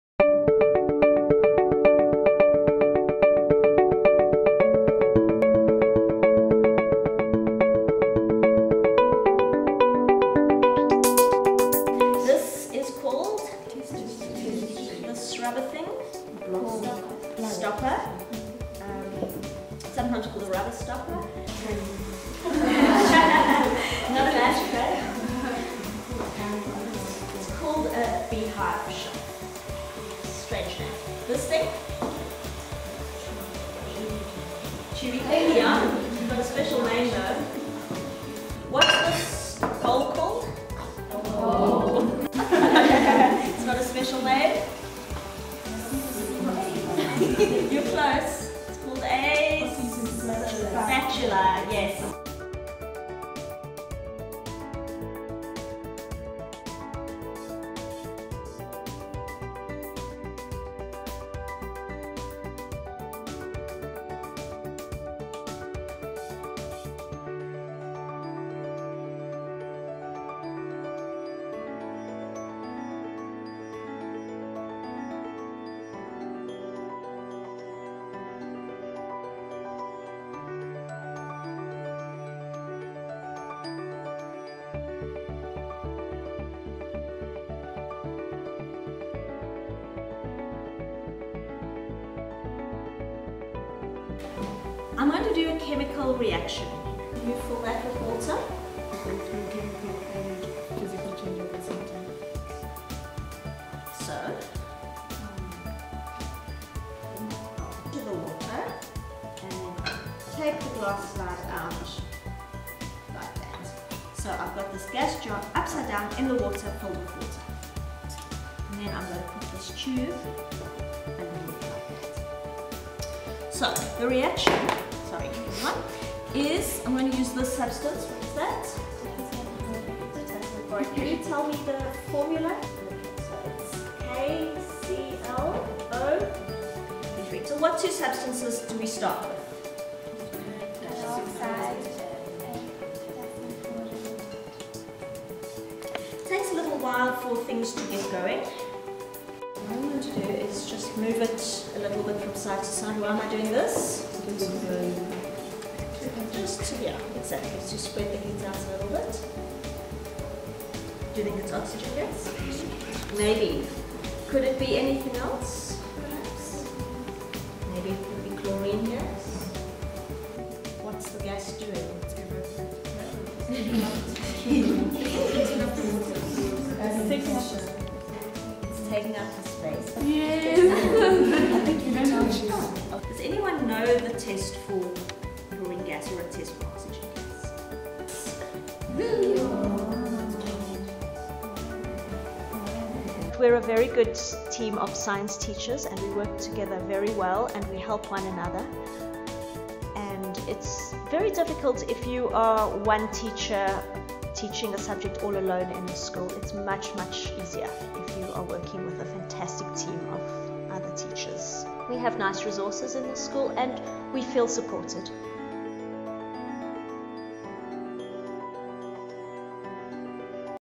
How Parktown girls got a science centre
Build it and they will come - that's proved true for a girl's school that, with help from Anglo American's Chairman's Fund, built a state-of-the-art science lab.